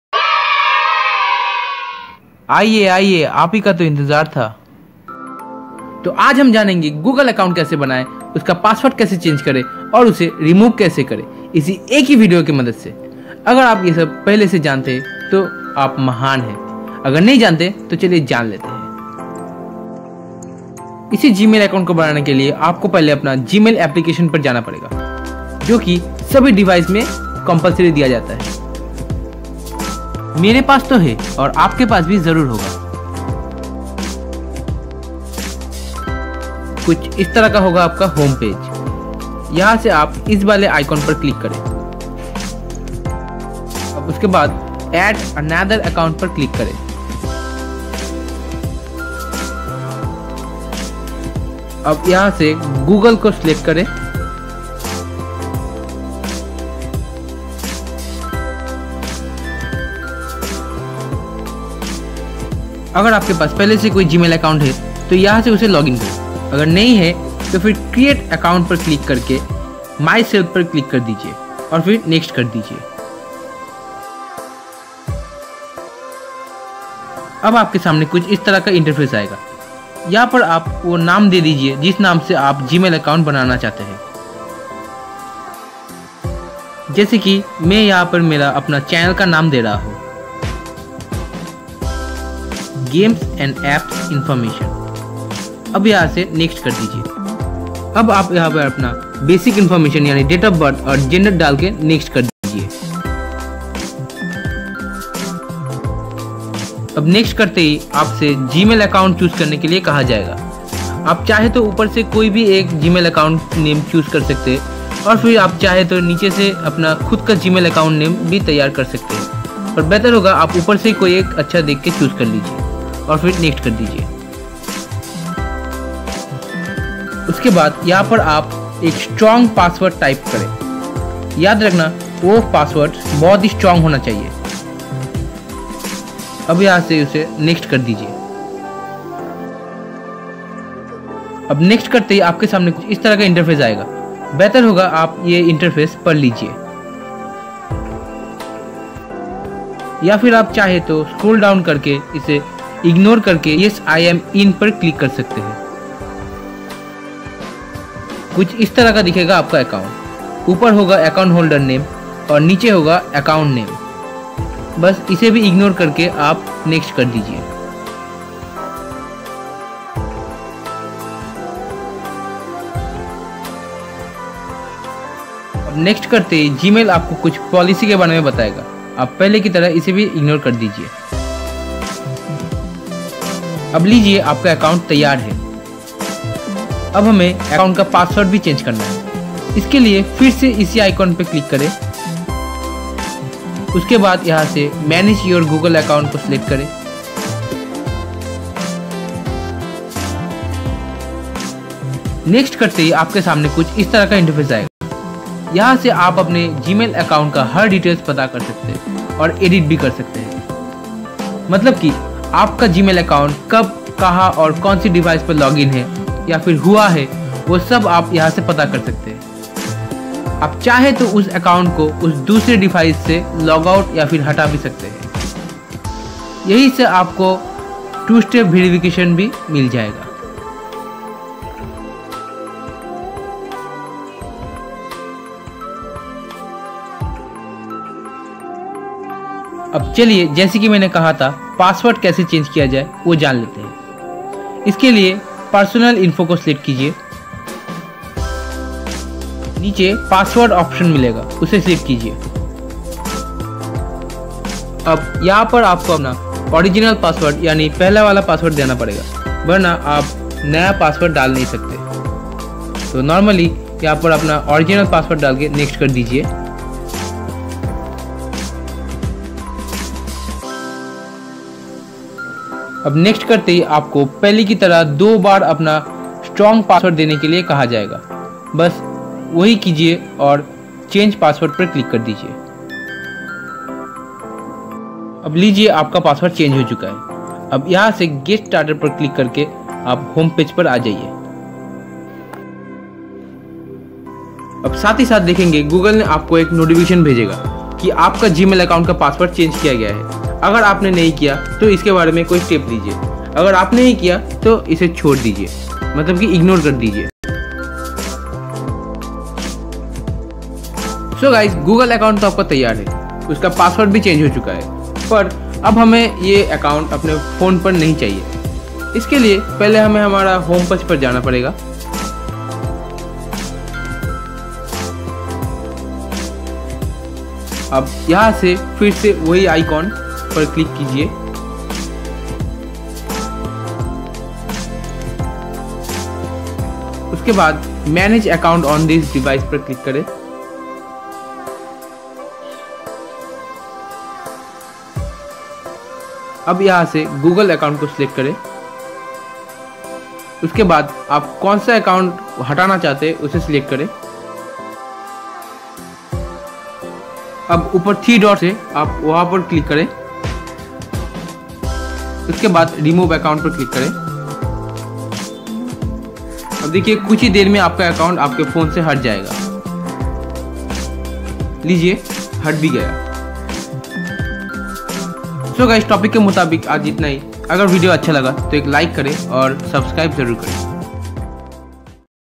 HOW TO CREATE, REMOVE AND CHANGE PASSWORD OF GMAIL ACCOUNT WITHIN 5 MINUTES || GAPPS INFO
gapps info
Games and application information